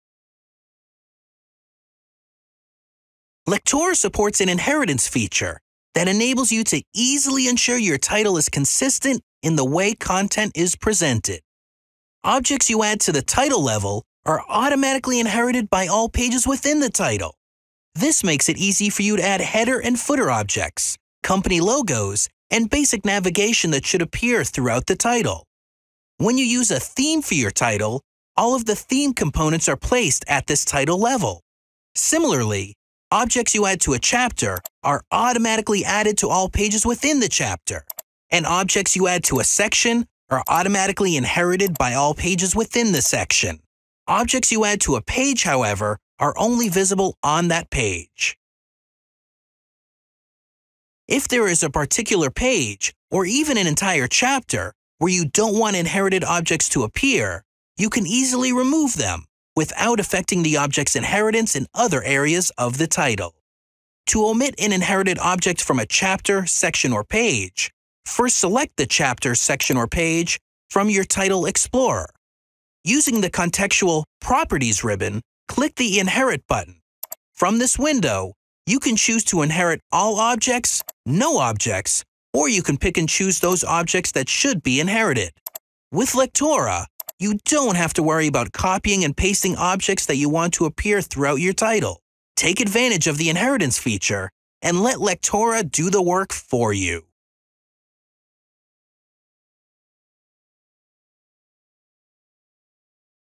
How to Use the Inheritance Feature in Lectora Inspire V11 e-Learning Software
Take advantage of the inheritance feature and let Lectora e-Learning software do the work for you. Our authoring tool helps ensure your e-Learning course is consistent from beginning to end.